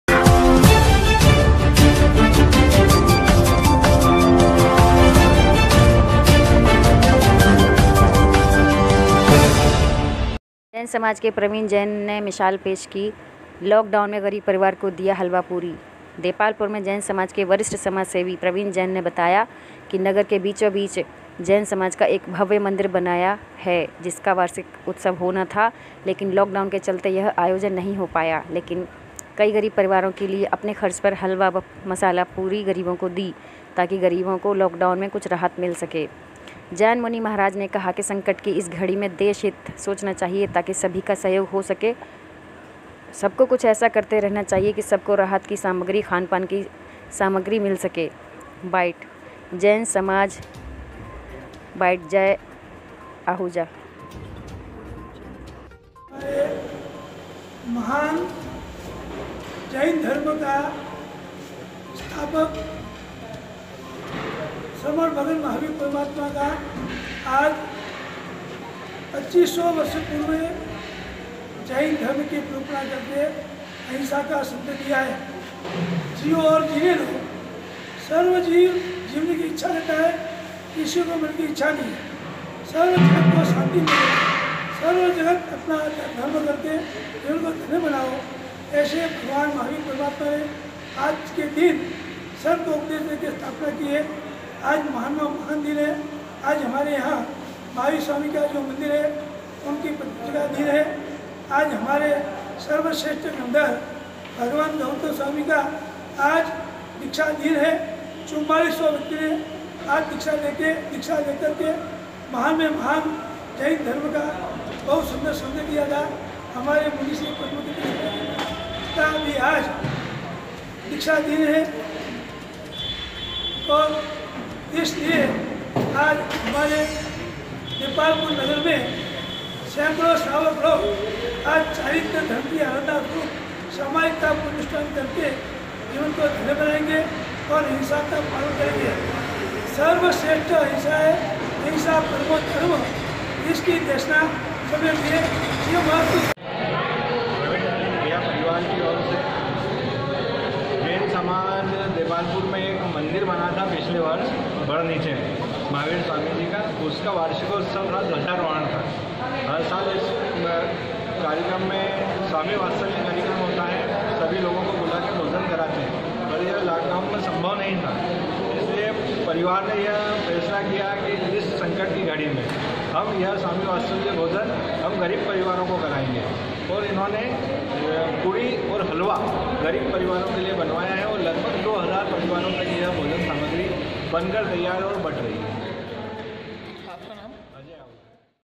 जैन समाज के प्रविण जैन की मिशाल पेश की लाकडाऊन मै गरीब परिवार को दिया हलवा पूरी
स्लग: // जैन समाज के प्रविण जैन की मिशाल पेश की   लाकडाऊन मै गरीब परिवार को दिया हलवा पूरी

एकर  देपालपुर मे जैन समाज के वरिष्ट समाज सेवी  प्रविण जैन   ने बातया की  नगर के बीचो बीच  जैन समाज का एक भव्य मन्दिर बनाया है जिसकी वार्षिक उत्सव होना था लेकिन लाक डाऊन के चलते  यह आयोजन नही हो पाया लेकिन काई गरीब परिवारो   कै लिए  आपने खर्च पर   हलावा व मसाला पुरी   गरीबो को दी  तकि  गरीबो को लोकडाऊन मे कूछ राहत मिल सके

जैन मूनि महाराज ने कहा की संकट की इस घडी मे  देश हित  सोचना चहिये तकि सभी का सहयोग हो सके  सब को ऐसा कुछ करते रहना चहियै की सब को  राहत की समाग्री खान पान की समाग्री मिल सके

बाईट  जैन समाज मासाहब

बाइट अजय आहुजा

देपालपुर से दिपक सेन कि रिपोर्ट